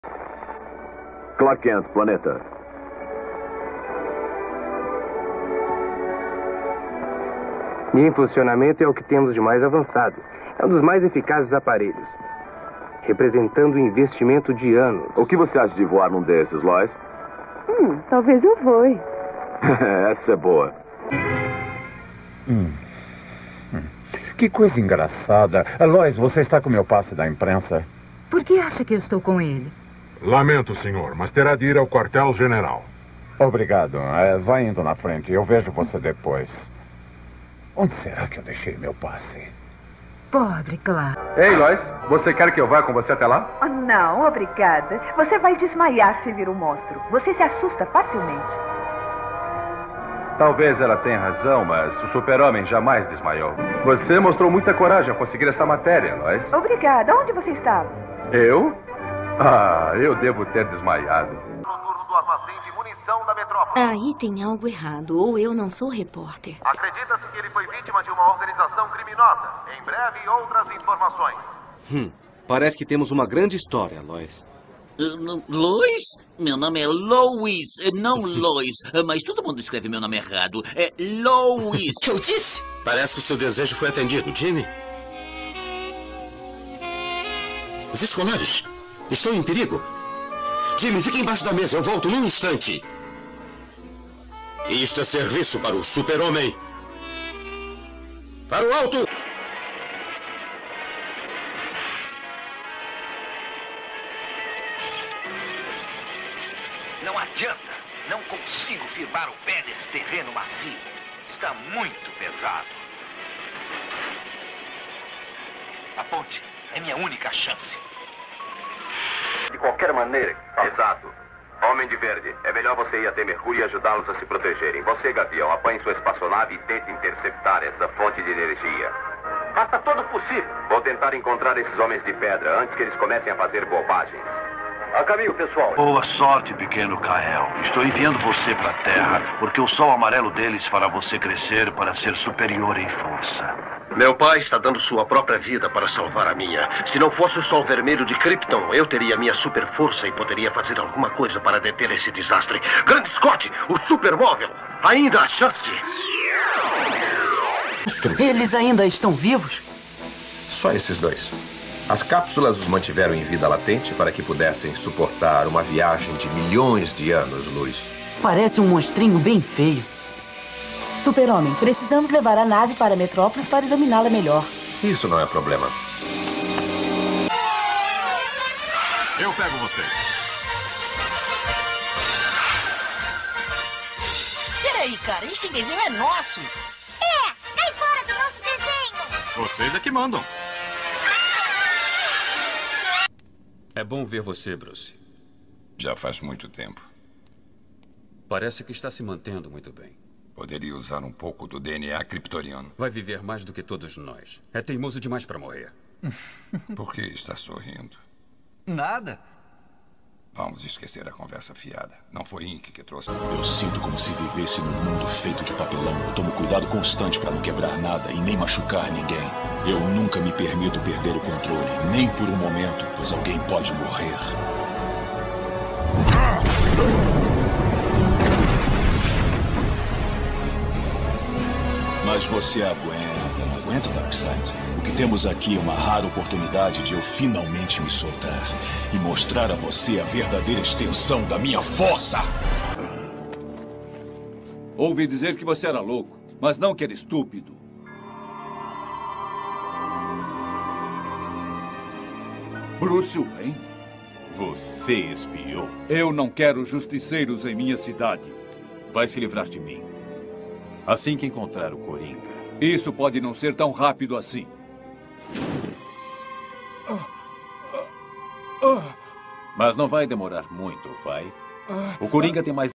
Superman - Todos os Dubladores Brasileiros
Todos os dubladores brasileiros do Superman (Super-Homem) nas animações.

Depois do Guilherme Briggs, o Reginaldo Primo, Raphael Rossatto e o Luiz Laffey chegaram a dublar o Superman em animações da DC.

Imagens meramente vinculadas para apreciação dos fãs.